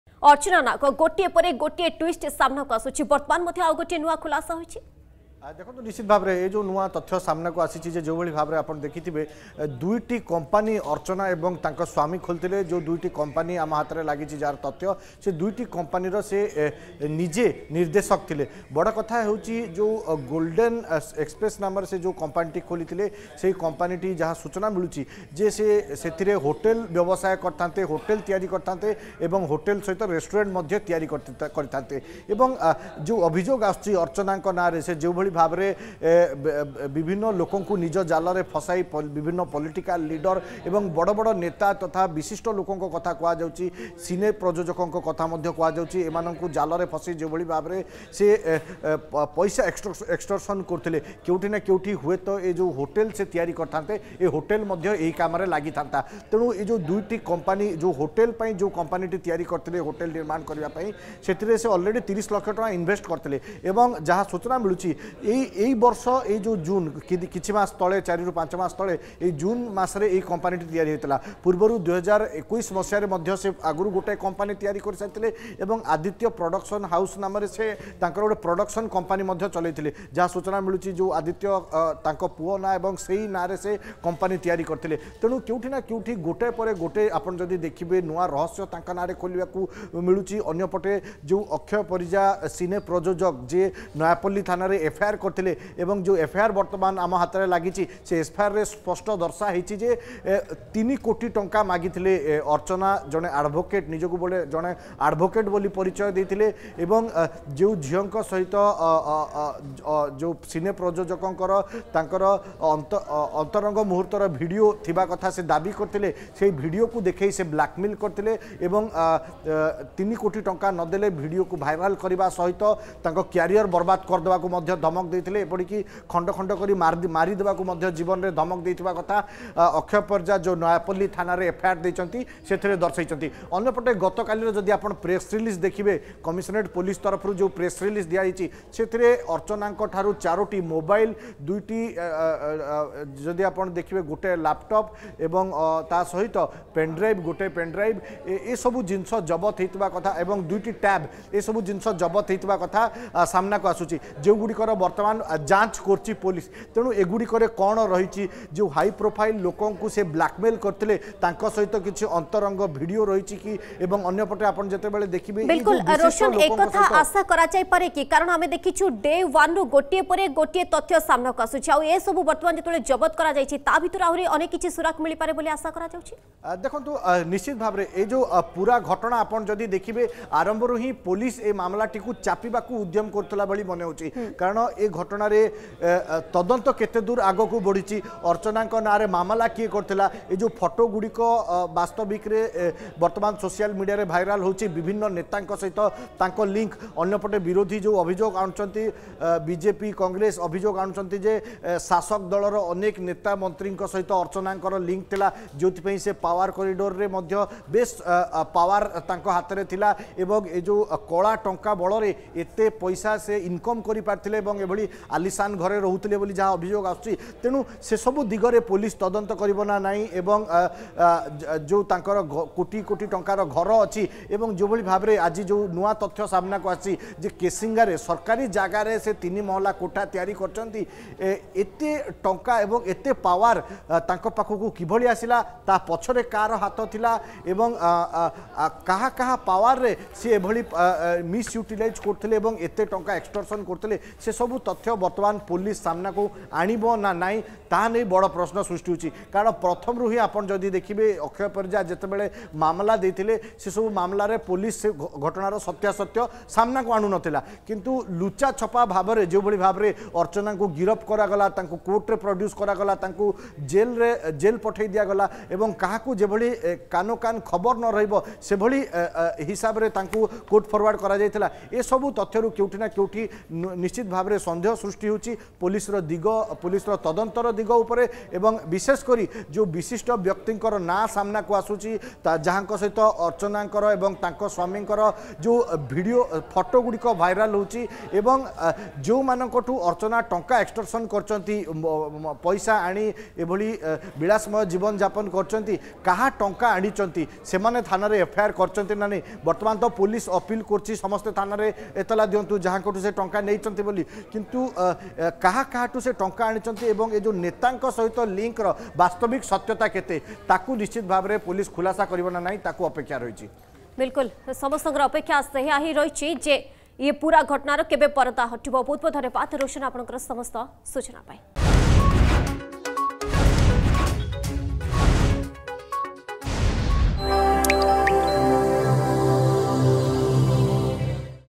ବ୍ଲାକମେଲର ଅର୍ଚ୍ଚନା ନାଗ ମାମଲାରେ ନୂଆ ଖୁଲାସା
ବ୍ଲାକ୍‌ମେଲର୍‌ ଅର୍ଚ୍ଚନା ନାଗ ମାମଲାରେ ନୂଆ ଖୁଲାସା । ହୋଟେଲ ସହ ରେଷ୍ଟୁରାଣ୍ଟ ତିଆରି କରିବାକୁ ଯୋଜନା କରିଥିଲେ ଅର୍ଚ୍ଚନା।